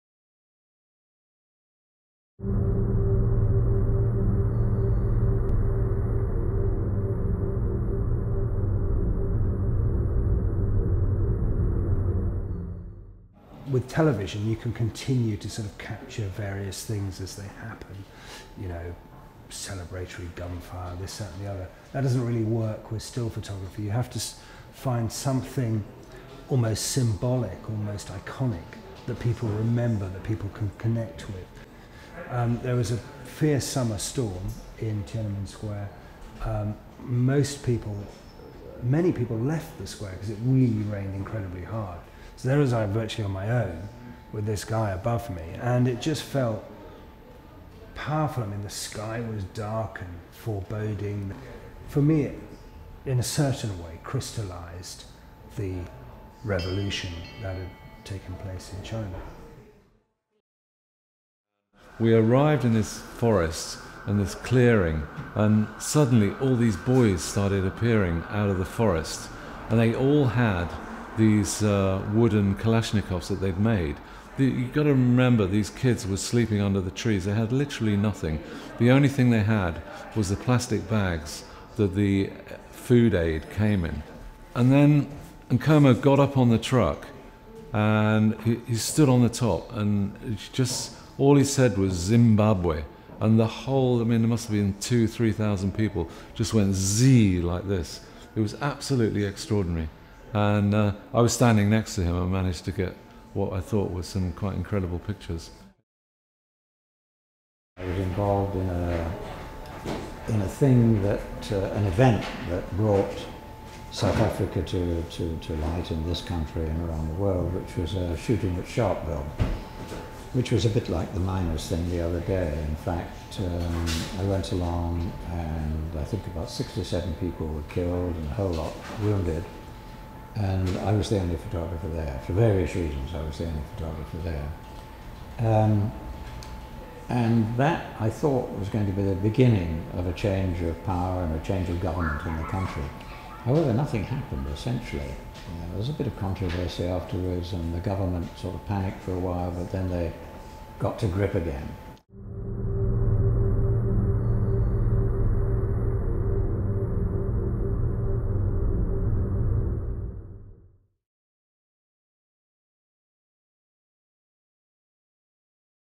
Magnum Revolution - 65 Years of Fighting for Freedom
Acclaimed Magnum Photographers Ian Berry, Stuart Franklin and Peter Marlow describe their work featured in Prestel's publication - Magnum Revolution, 65 Years of Fighting for Freedom.